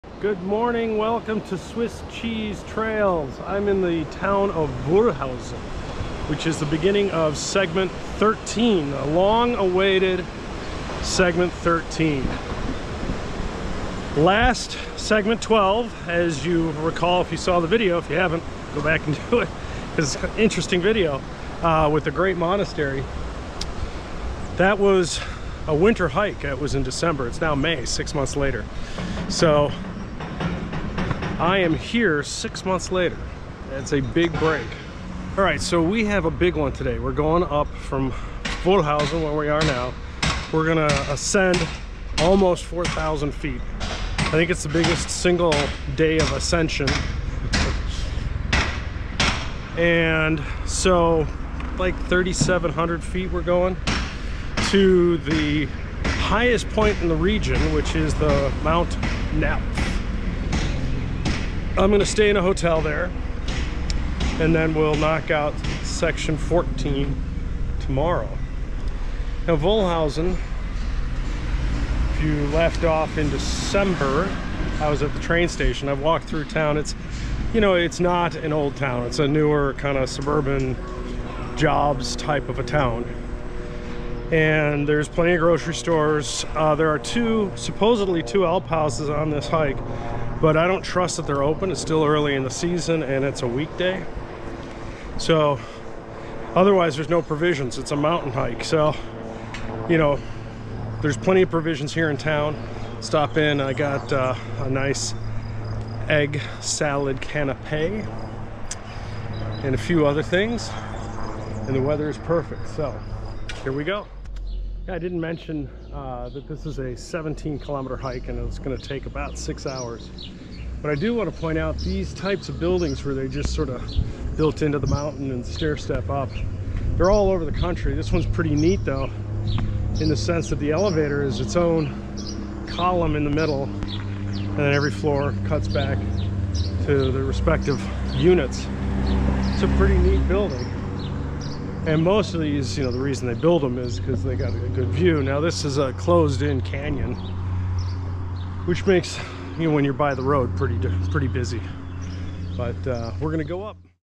Start of Stage 13; Wolhusen to Napf Mountain, Swiss Alpine Panorama Trail (Daily 360° VR Video)
Swiss Alpine Panorama Trail, Segment 13, #1 - Wolhusen to Napf Mountain. Before hiking this section, I knew it was going to be challenging, I knew the weather and season were going to determine when I could hike and I knew that the views were going to be some of the best views on the whole trail. I really wanted to make this hike memorable. I waited until the weather was ideal and hopefully my 360° videos will attest to this fact.

Length: 12.5 miles (17 km);
Hiking time: 6.5 h;
Ascent: 3,775 ft (1150 m);
Descent: 1,000 ft (300 m);
Fitness Level: Difficult.

Find the best VR experiences by searching for Swiss Cheese Trails on Oculus TV.

You can find more 4K, 360° YouTube videos of me with my son, Ryan, in the USA at Hike360